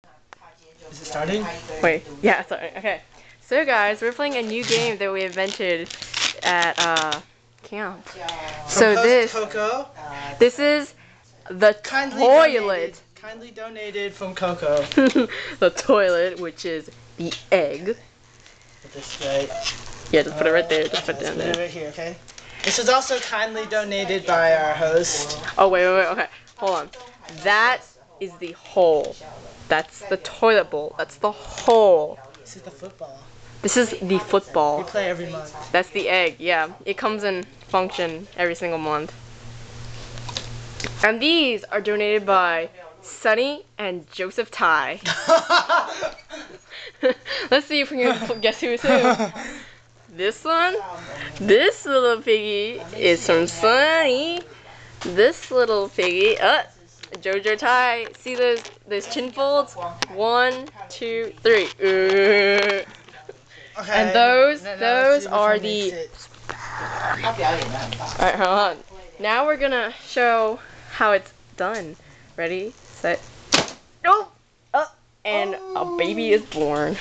Tadpole Toilet Toss
We're shooting tadpoles through a toilet seat to hit that football. Nothing more.